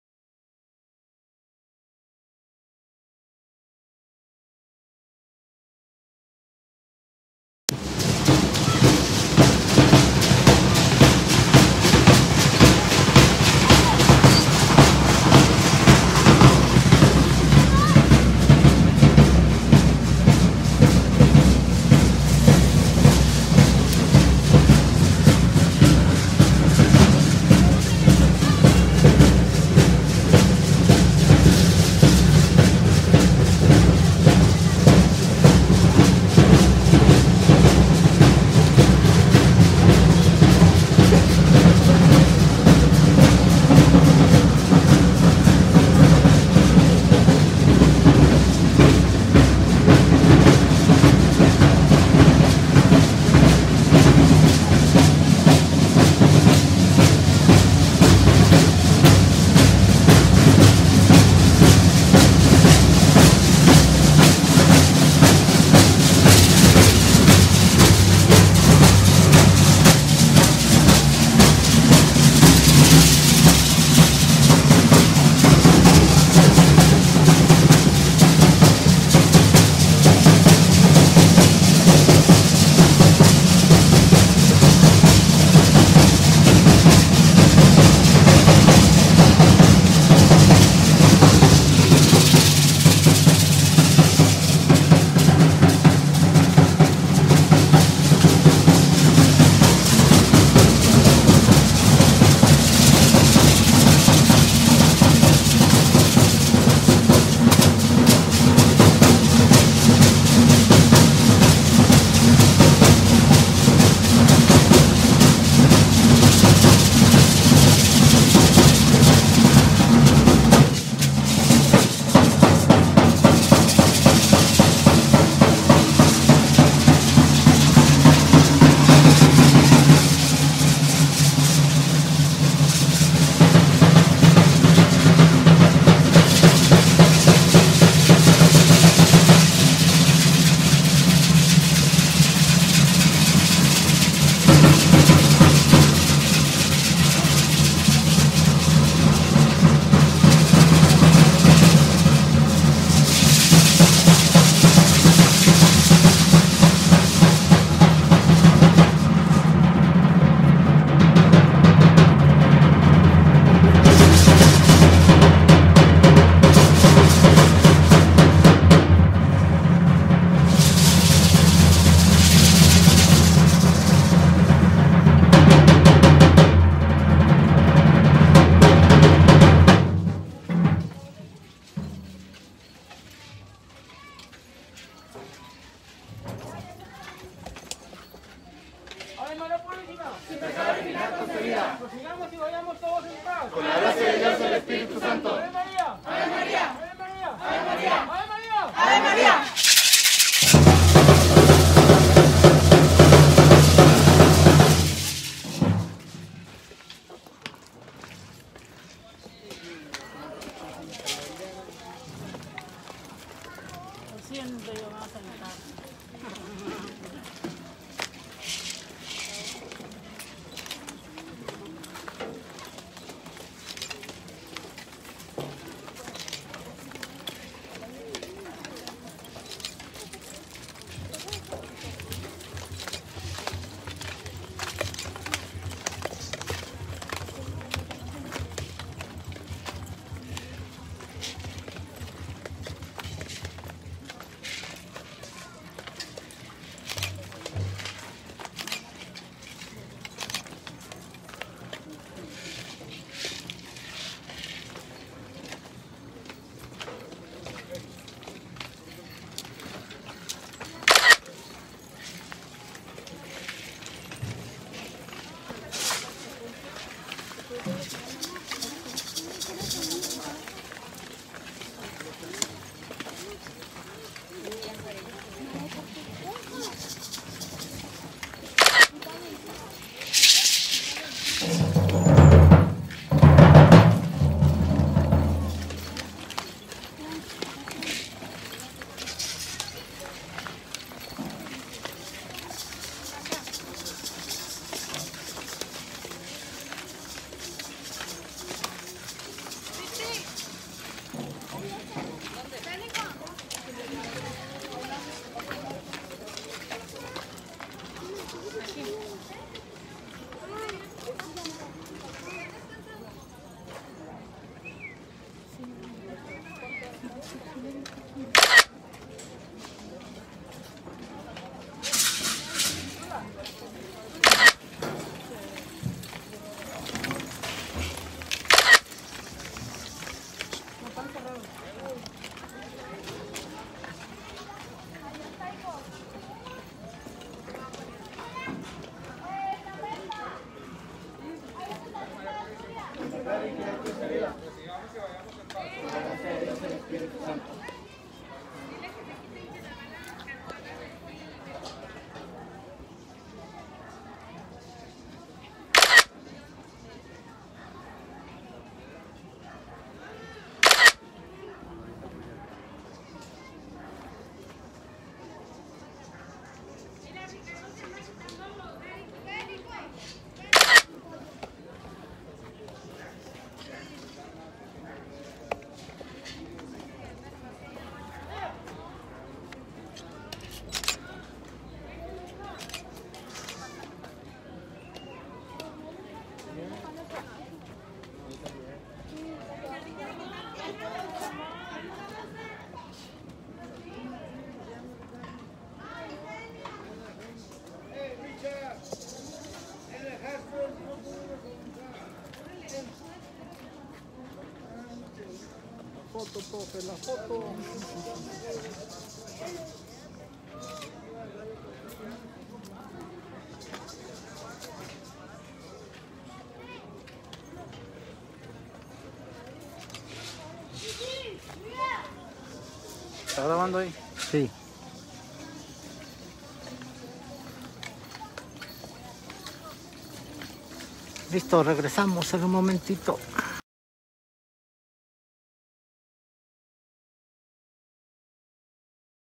Llegando a La Enramada "Manto Azul Celeste" y "Los Cuates"
Bendición de Estandarte y Danza "Manto Azul Celeste" de La Col. Luis Echeverría de Torreón, Coah. el 3 de octubre de 2020. Padrinos Danza "Los Cuates"